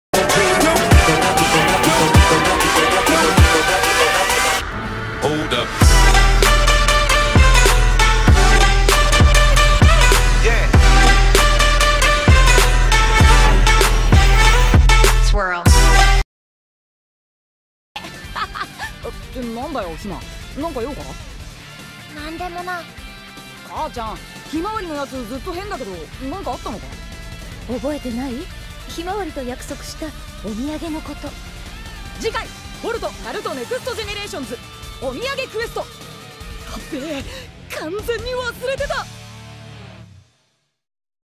Boruto Naruto Next Generations Episode 32 Review
Sup, Again with another Exciting Review Vodeo. This time it's Boruto Naruto Next Generations Episode 32. Boruto forgot to get her sister a gift and finally realises. He is now trying to get the perect gift for her. Was I the only one who saw Boruto's Byakugan... It just an Instant, but it was Awesome. Don't Forget to Like and Subscribe. Thanks For Watching